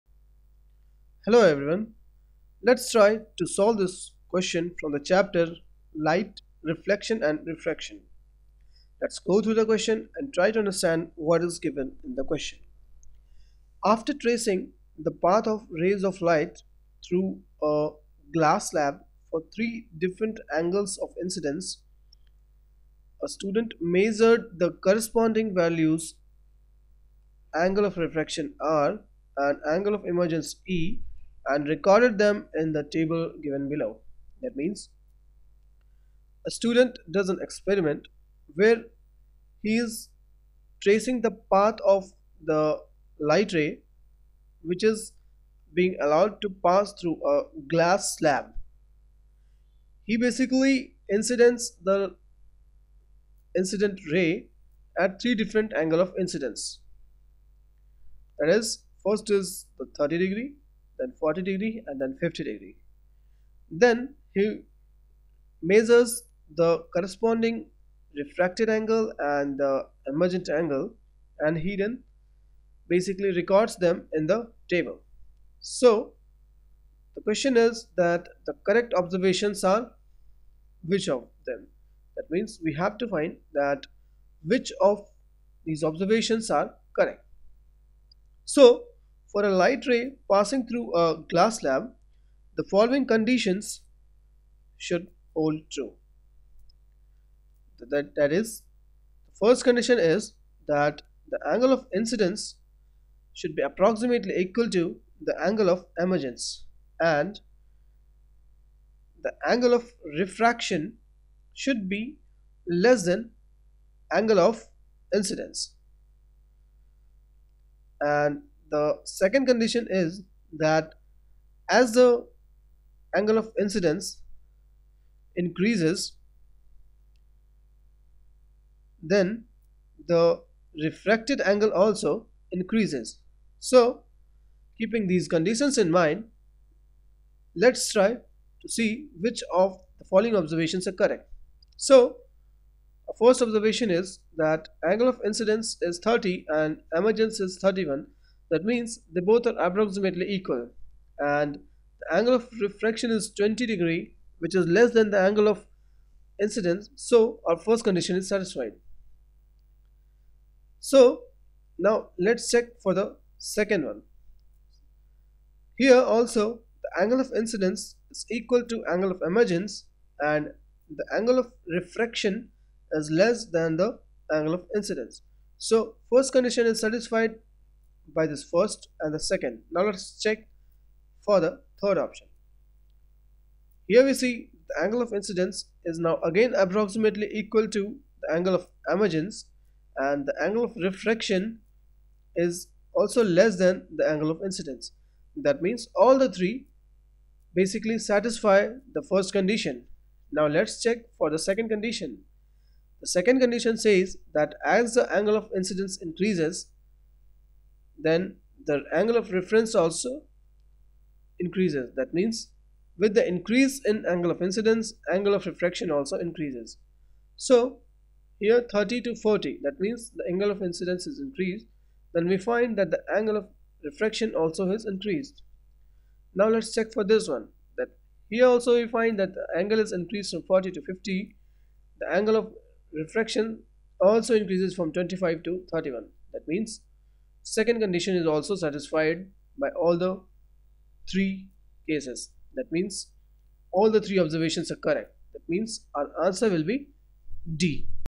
CBSE Board Papers Class 10 - 2013 - Physics - Question 30
Question - After tracing the path of rays of light through a glass slab for three different angles of incidence, a student measured the corresponding values angle of refraction r and angle of emergence e and recorded them in the table given below:
The correct observations are:
A. I and II
B. II and III
C. I and III
D. I, II and III